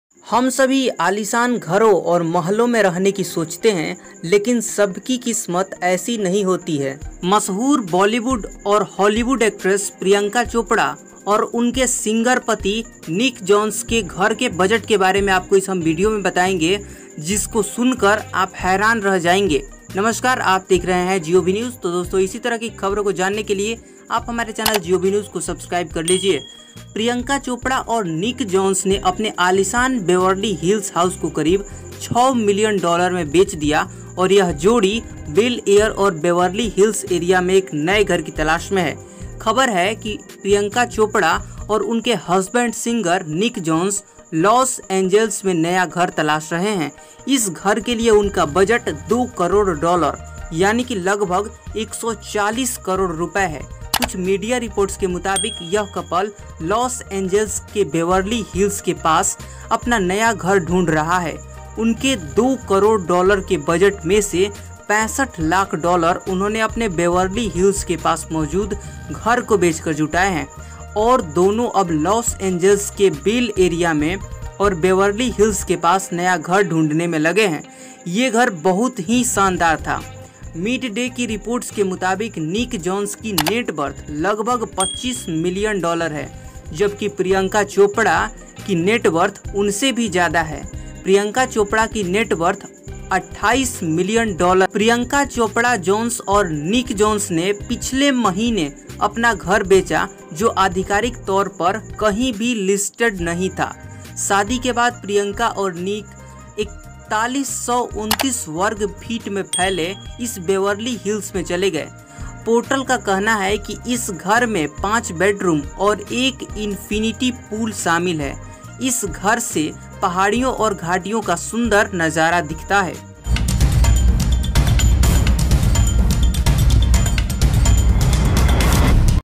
Priyanka Chopra और Nick Jonas के घर की कीमत जानकर हैरान रह जाएंगे | Expensive house | govnews
Priyanka Chopra and Nick Jonas sold their stately Beverly Hills house for about $ 6 million, and the pair will sell a new ...